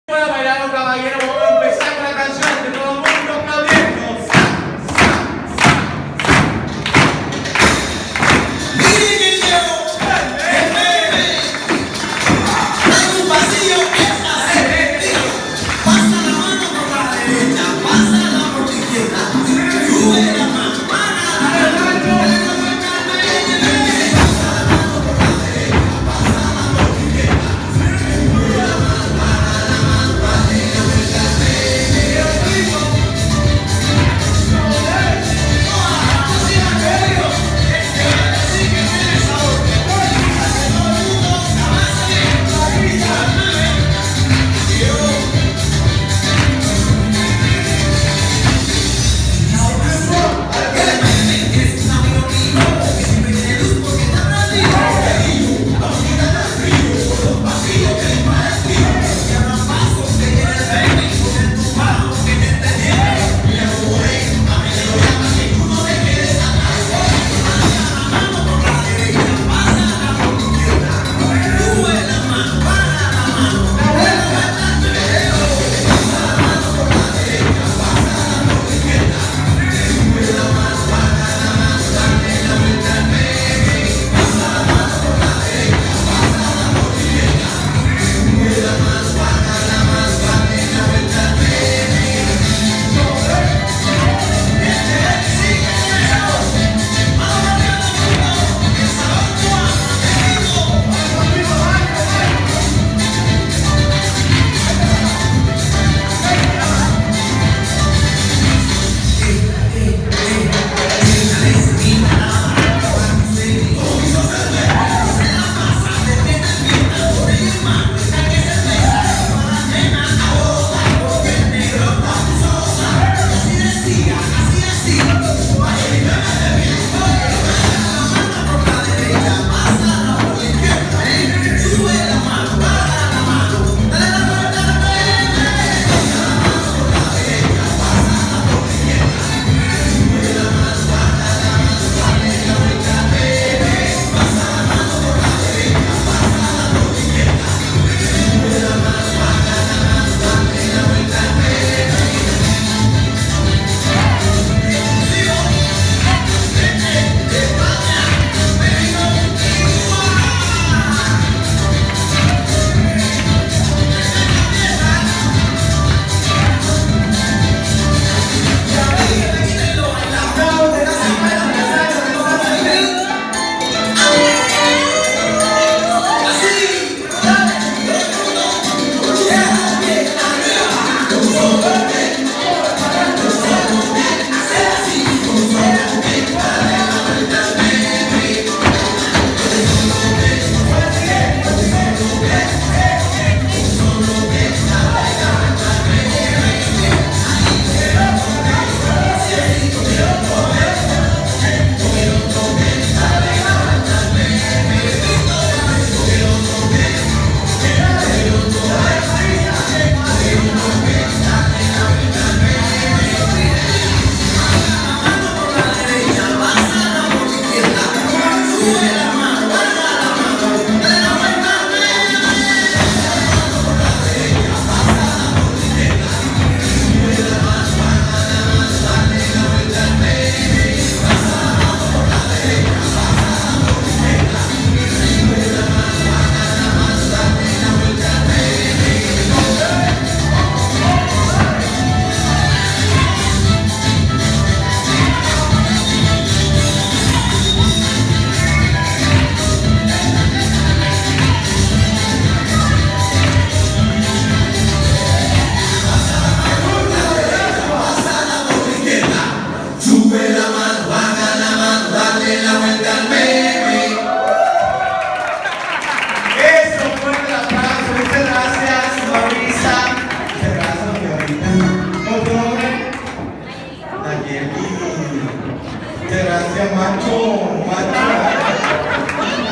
Josie Line Dancing in Mazatlan
Josie dances in a line dance in Mazatlan.